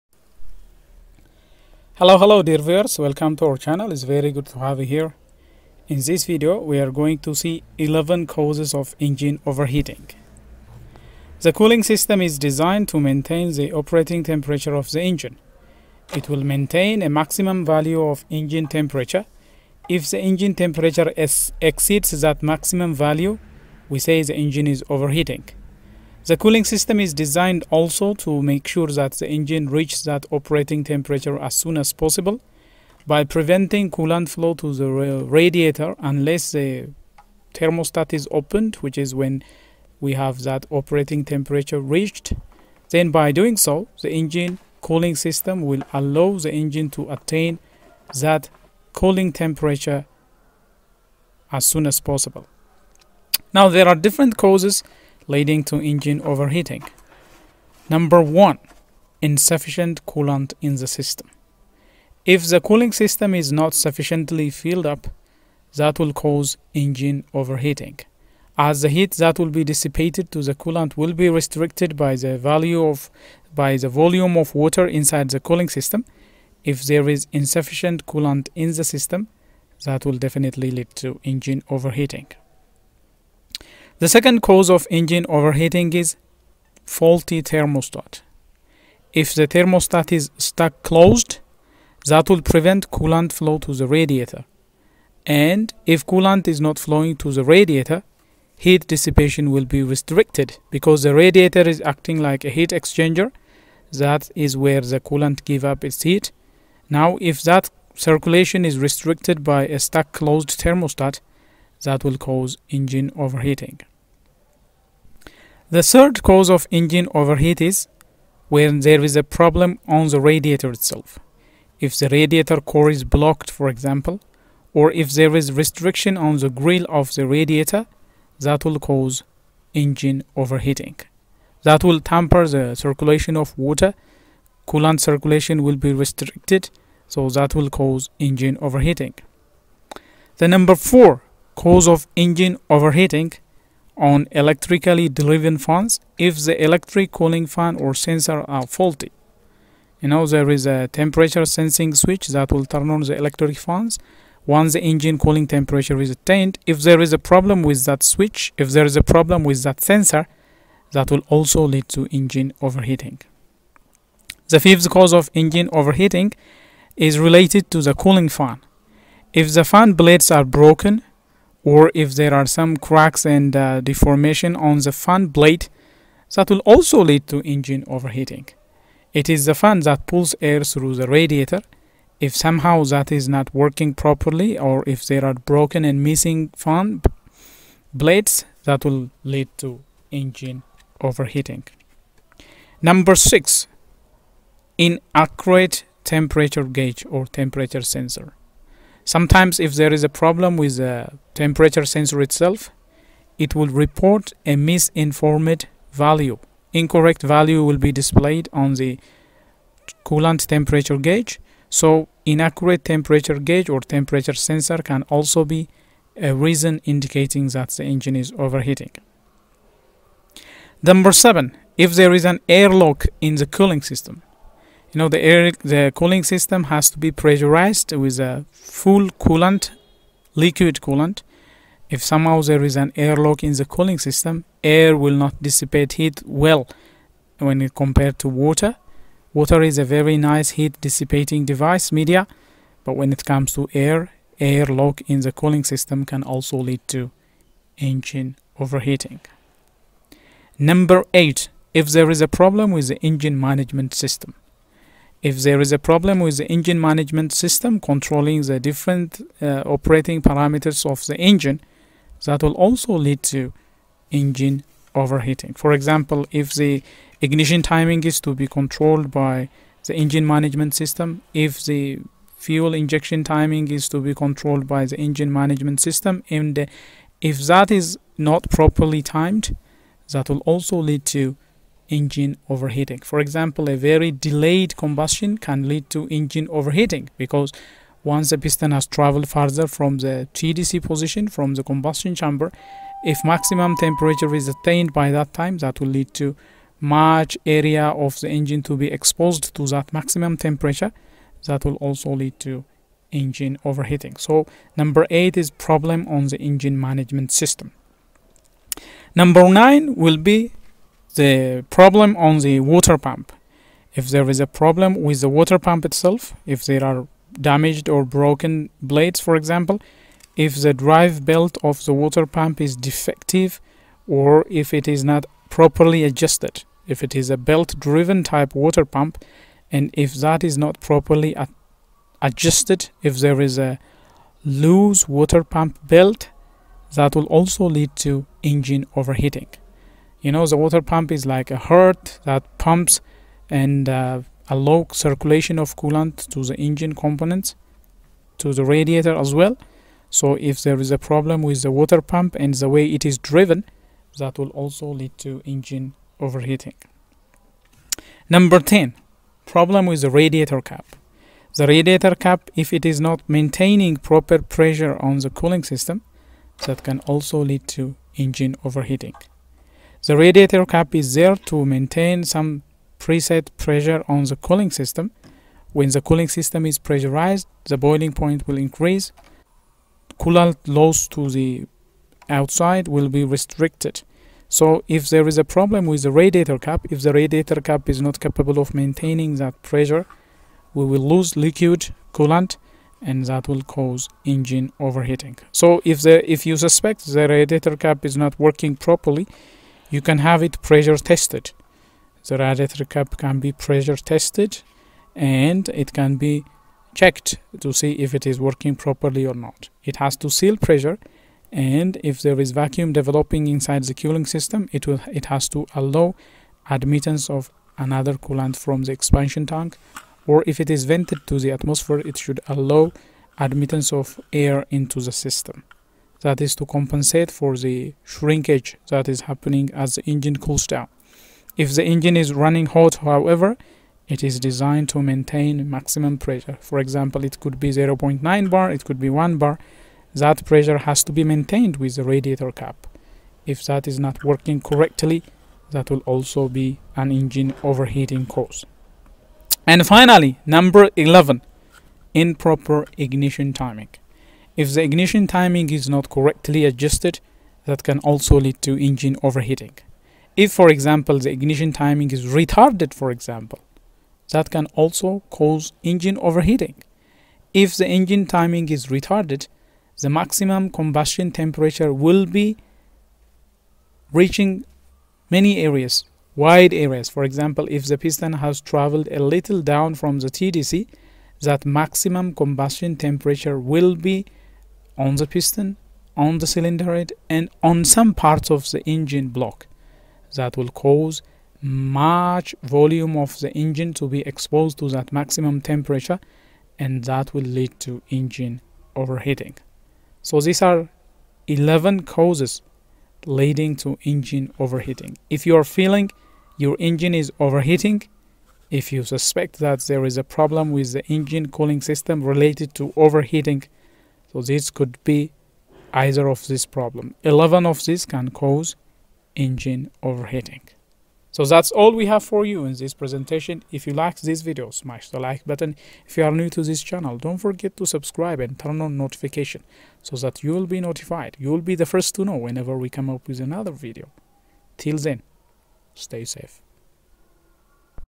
11 causes of engine overheating
In this video we will look at 11 things leading to engine overheating. Car engine overheating can be a frustrating experience specially if you couldn't figure out what is causing the problem. This video is prepared to help you pin point the cause of engine overheating.